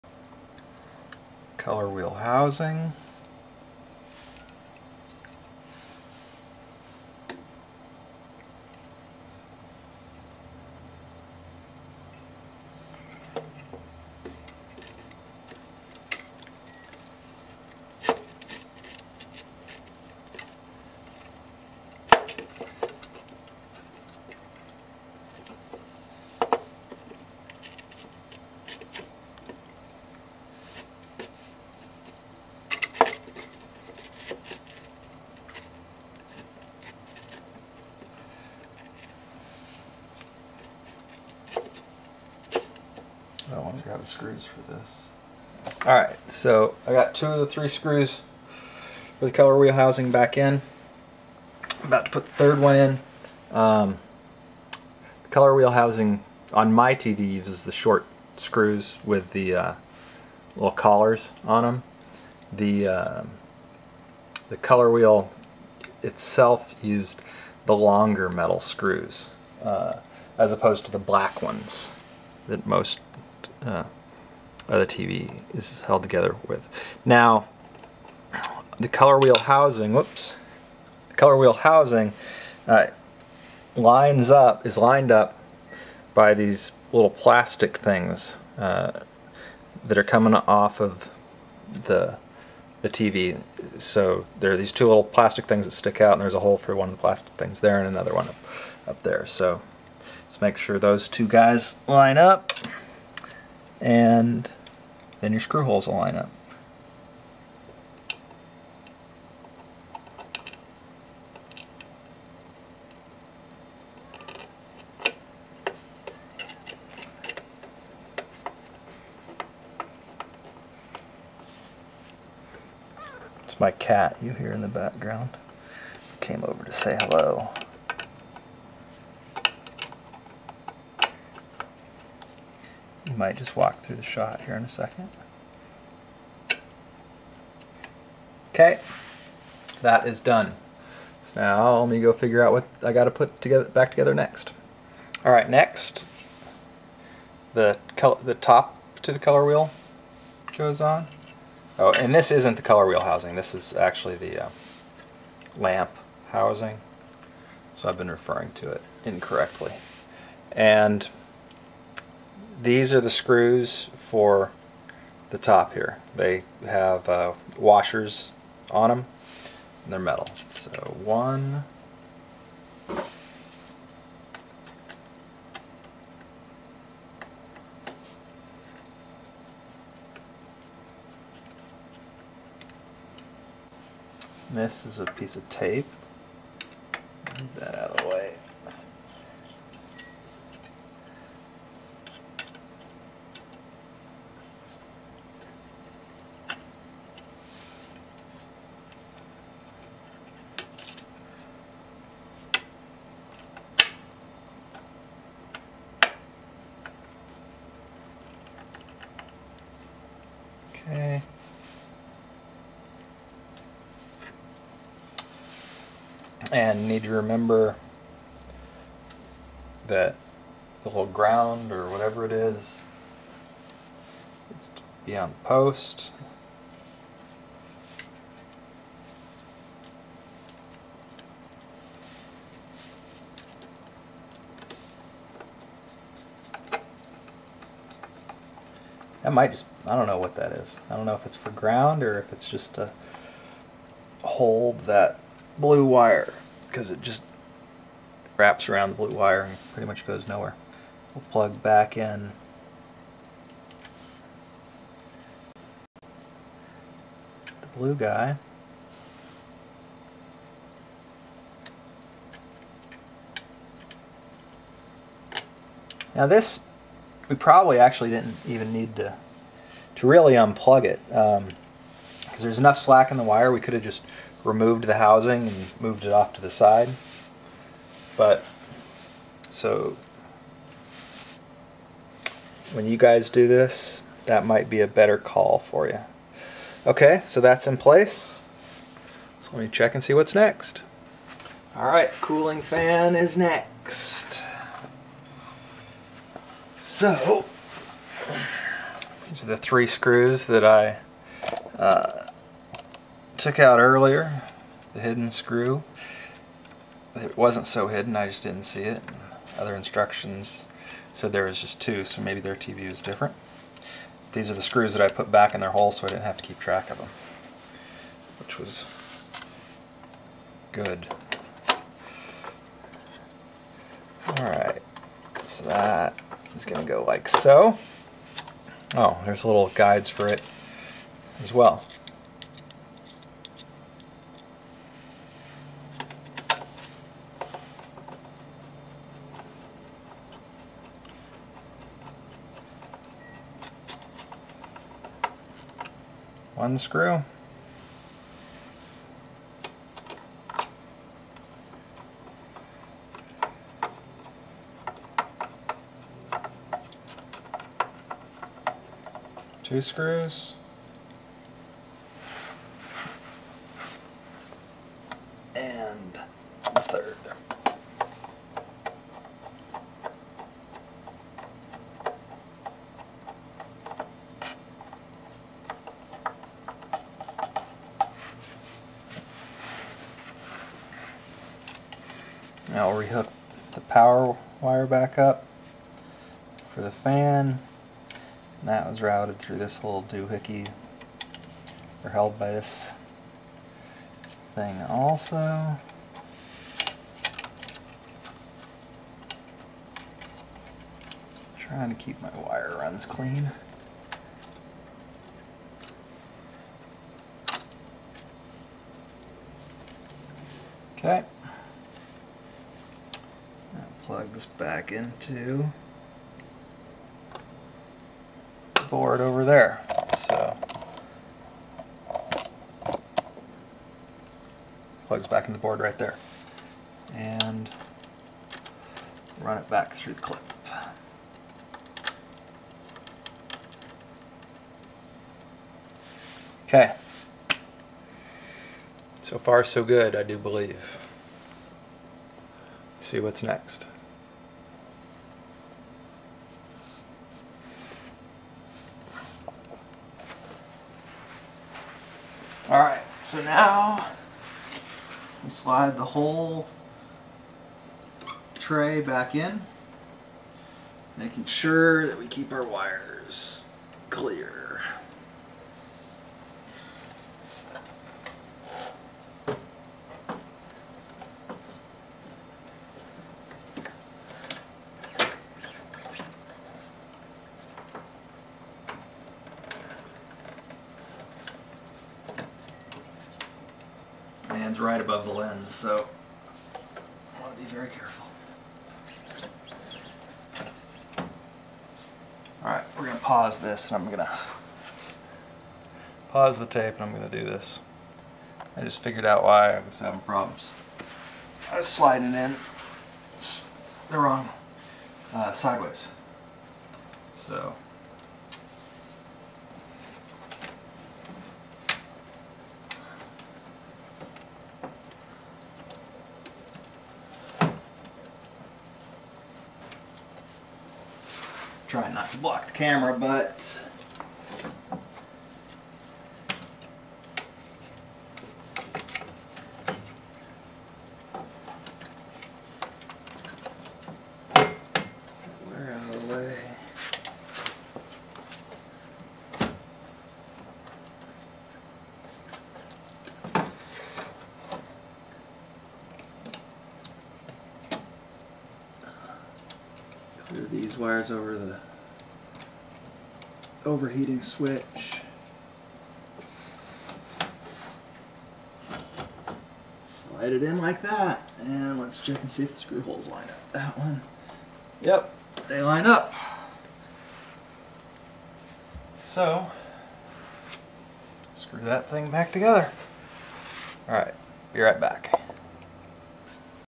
Replacing the color wheel in a Samsung DLP Television Part 4
PART 4

I had to replace the color wheel in my Samsung DLP television. It was pretty easy to do and I shot video of the proceedure in the event others wanted to see how it is done.